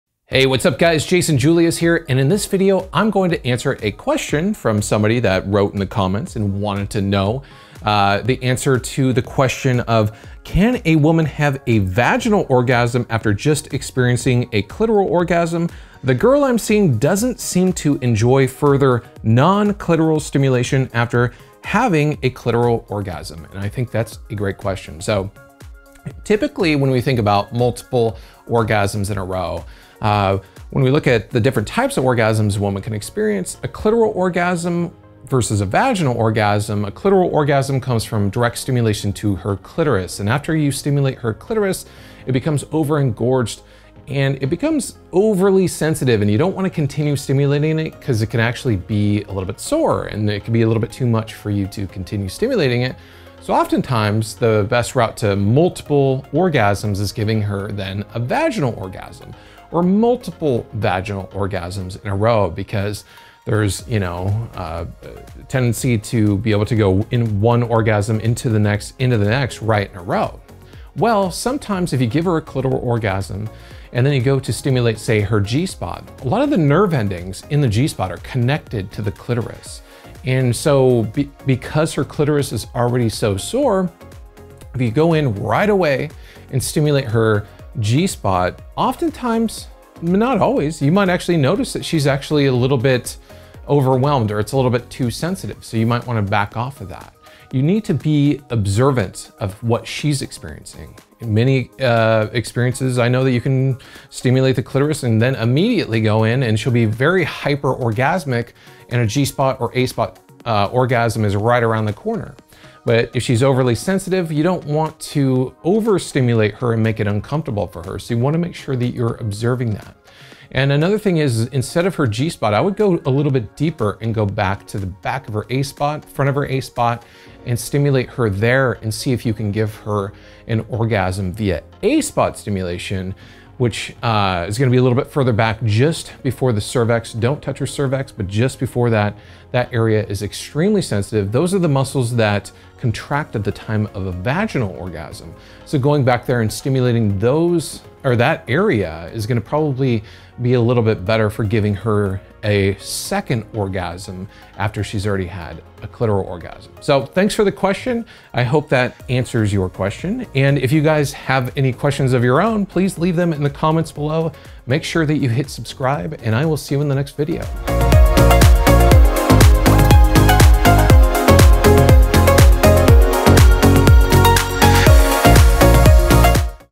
Vaginal Orgasm After Clitoral Orgasm?
In today's video I want to answer a question from a viewer about whether you can give a woman a vaginal orgasm immediately after a clitoral orgasm. This is a great question, and I feel like it comes from a good place from someone who genuinely cares about making sure his woman has the best experience possible.

When we think about multiple orgasms we need to make the distinction between clitoral orgasms and vaginal orgasms. Clitoral orgasms come from direct stimulation to a woman's clitoris which is also the most common way a woman stimulates herself to orgasm via masturbation. A vaginal orgasm come from direct stimulation to the inside of her vagina via either her G-Spot or A-Spot.

One very important distinction is that after you give a woman a clitoral orgasm her clit will become over sensitive and almost sore making it very difficult to give her multiple orgasms. With vaginal stimulation a woman can have multiple orgasms.

In many cases it's great to start with a clitoral orgasm to get her hyper orgasmic and ready for multiple vaginal orgasms. And in many cases a G-Spot or A-Spot orgasm is just around the corner. However, for some women it can be too much to stimulate the inside of their vagina immediately after a clitoral orgasm.

The reason is there is a lot of nerve endings in the G-Spot that are connected to the clitoris that is already over engorged and sore from the previous stimulation. The best thing you can do is pay attention to her body language and how she's responding to the type of stimulation you're giving her.

You might try stimulating her A-Spot as it's a little deeper. This might allow her to have another orgasm sooner after clitoral stimulation. But always make sure to pay attention to her body language as you stimulate her to understand if she's enjoying the experience.

If you want to learn how to stimulate her G-Spot and understand how natural it is to give women amazing squirting orgasms you can go check out my purely educational video out over on my blog